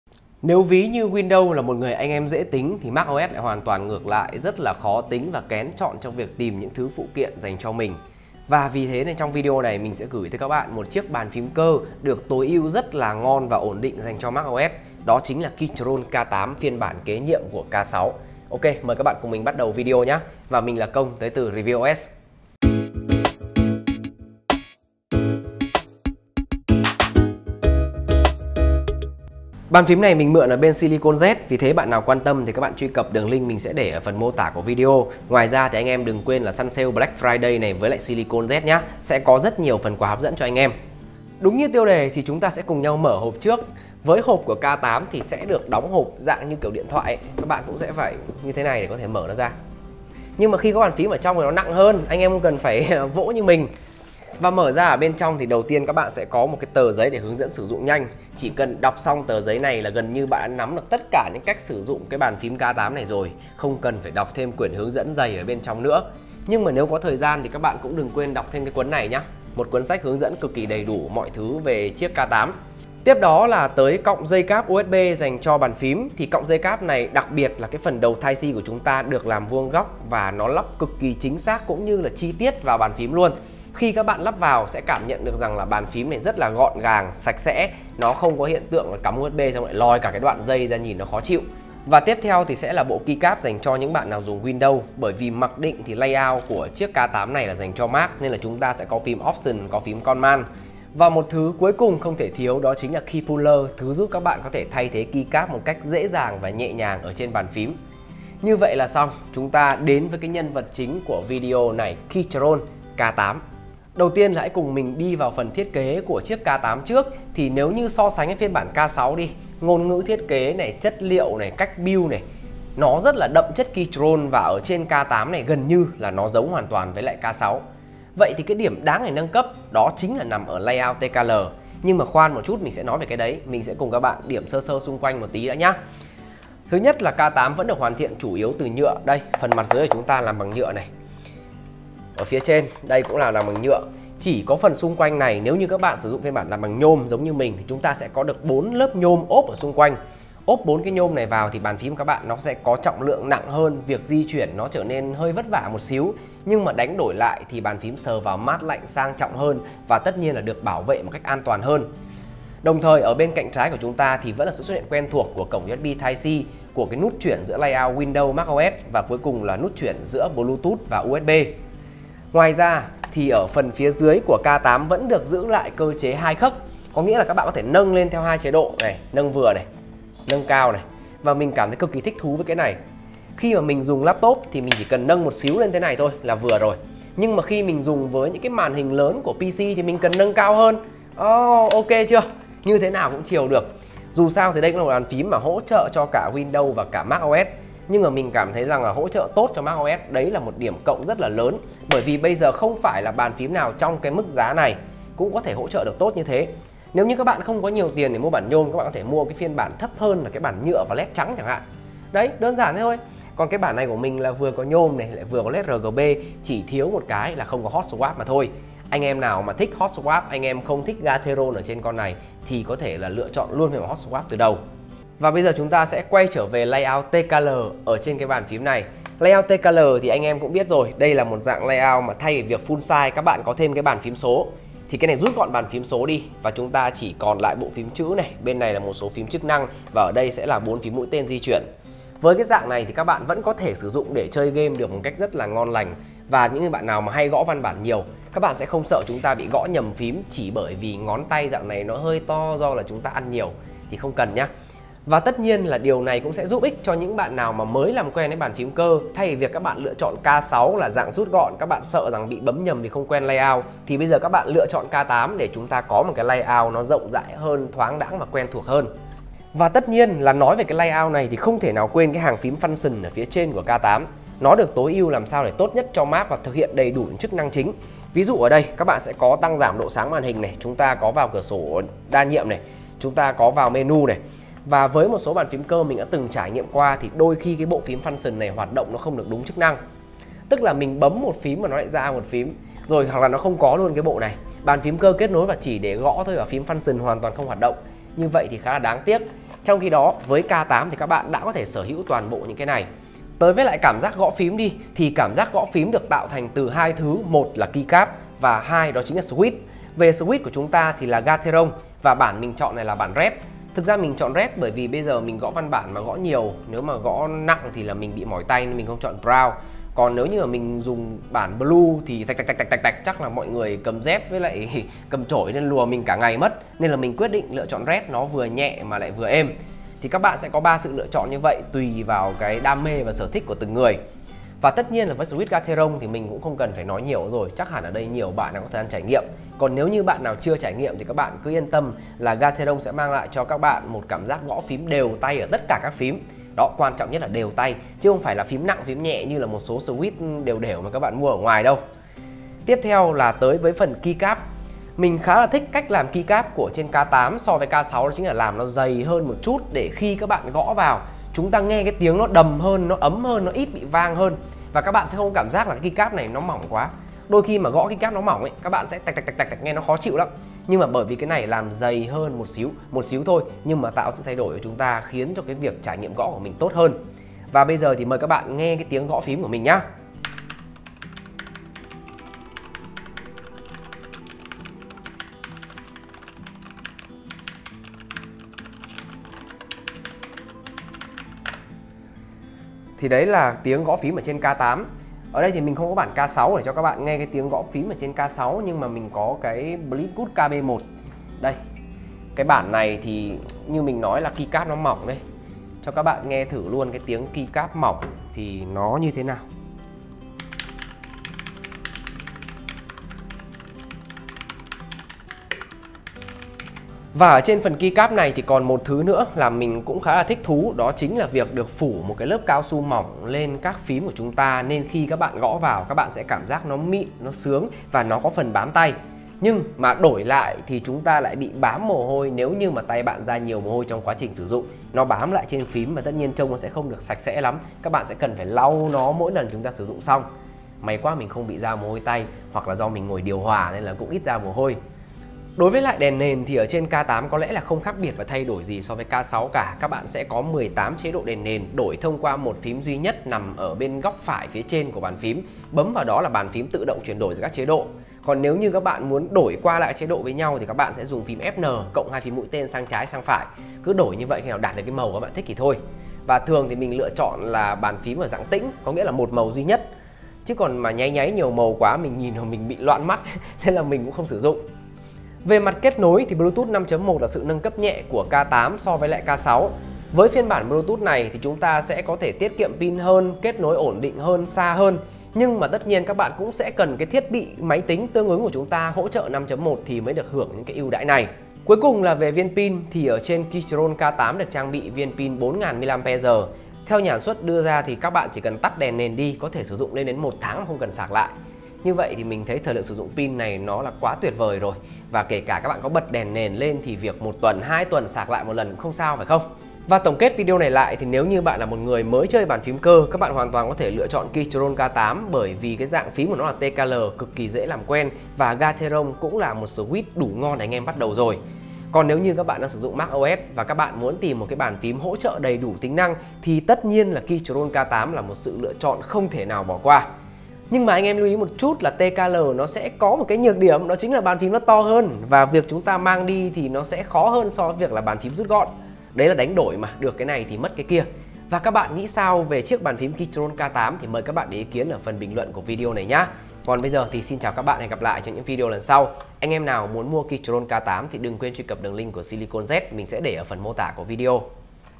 Mở hộp và trên tay Keychron K8: Bàn phím cơ bluetooth tốt nhất cho MacOS
Keychron K8 sở hữu thiết kế TKL dễ dàng làm quen kể cả với những người mới chơi bàn phím cơ bluetooth. Thiết kế đậm chất Keychrone là thứ không thể thiếu trên K8 cộng thêm một vài điểm cải tiến nhỏ nhưng rất chiều lòng người dùng. Với Keychrone K8 hãng đã thể hiện sự quan tâm sâu sắc tới trải nghiệm người dùng khi thay đổi từ 65% sang TKL, keycap dày hơn tiếng gõ ấm hơn. Chi tiết mời các bạn cùng ReviewOS theo dõi tại: "Mở hộp và trên tay Keychron K8: Bàn phím cơ bluetooth tốt nhất cho MacOS".

Mọi bản quyền thuộc về ReviewOS Channel. Vui lòng không sao chép dưới mọi hình thức.